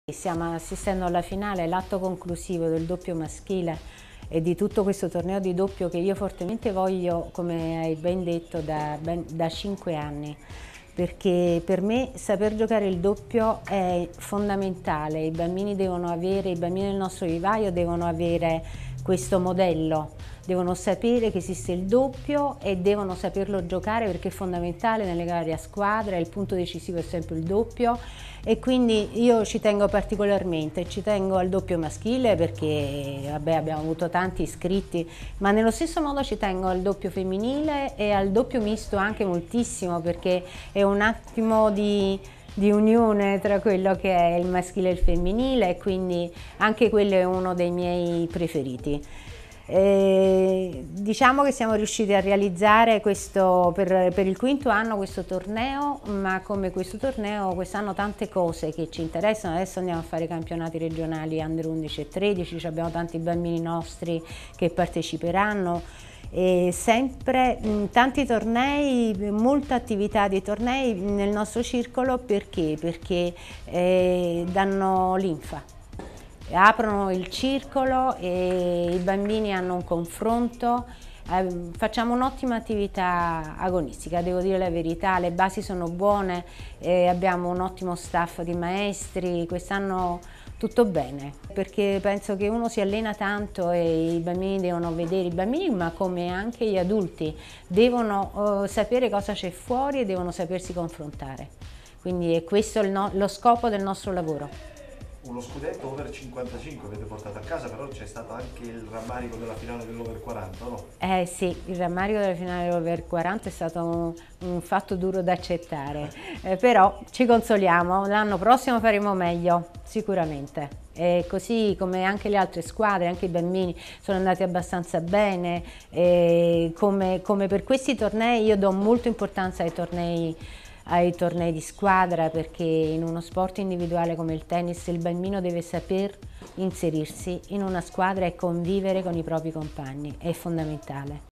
Stefania Crimaldi a ST: "Così intendiamo il tennis al TC Petrarca"
Parla ai microfoni di SpazioTennis Stefania Crimaldi, presidente del TC Petrarca. Ecco l'intervista realizzata da Silver Mele e Gianluca Miranda.